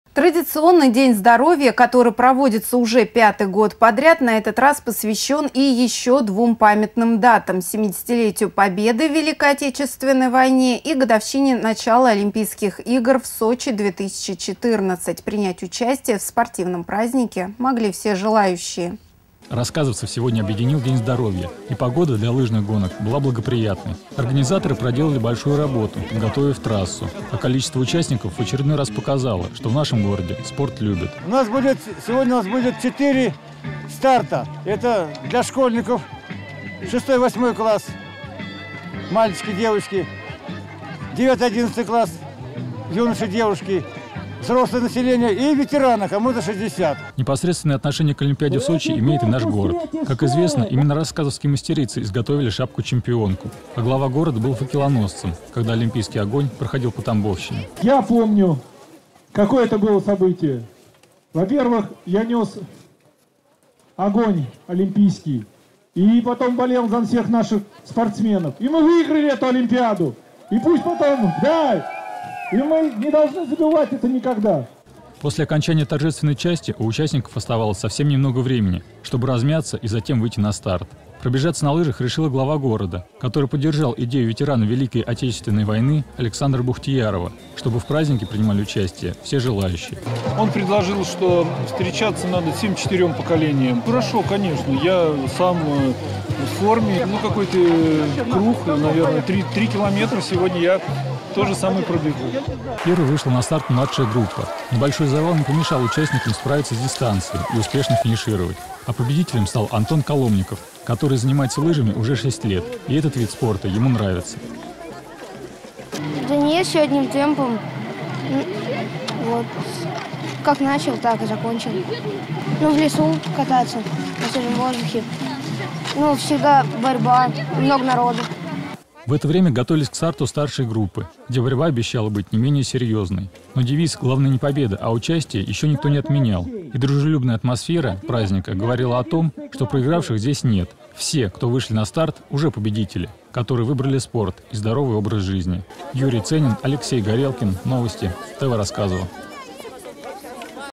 Лыжные гонки в «День здоровья»
Традиционный «День здоровья», который проводится уже 5 год подряд, на этот раз посвящен и еще двум памятным датам – 70 – летию победы в Великой Отечественной войне и годовщине начала Олимпийских игр в Сочи 2014. Принять участие в спортивном празднике могли все желающие.

Юрий Ценин